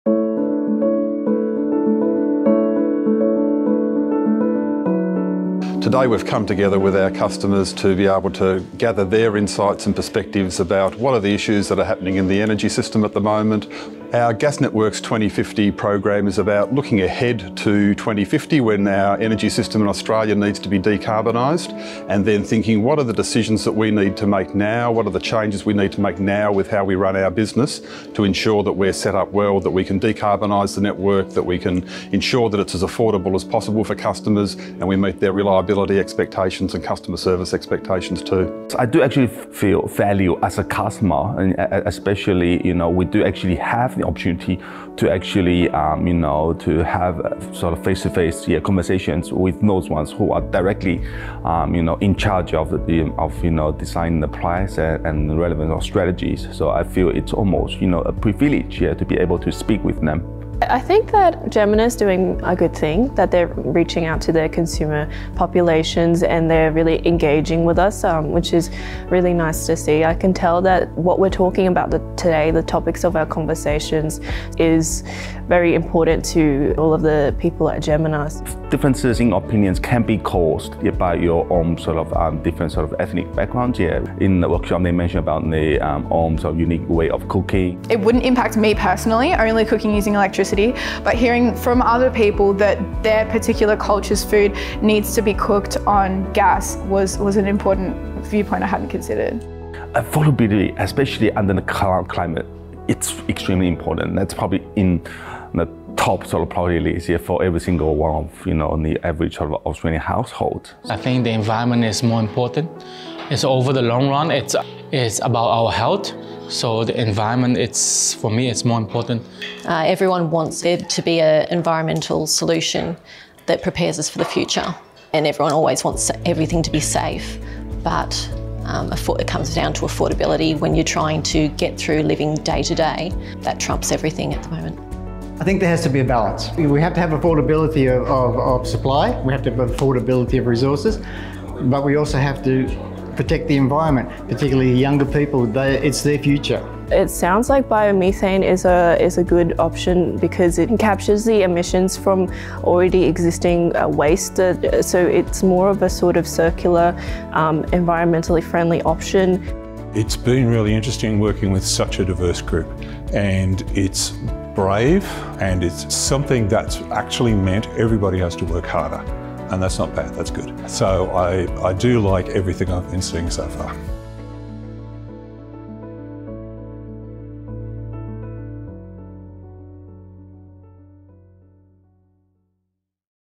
Gas Networks 2050: Hearing from our customers and Jemena's Shaun Reardon
The role of energy is changing and our Gas Networks 2050 program is about collaborating with our customers, discussing the challenges and looking ahead to when Australia needs to decarbonise. Hear what Shaun Reardon,  Executive General Manager, Jemena Networks, and our customers had to say and how they are collaborating with us on our plans for the future.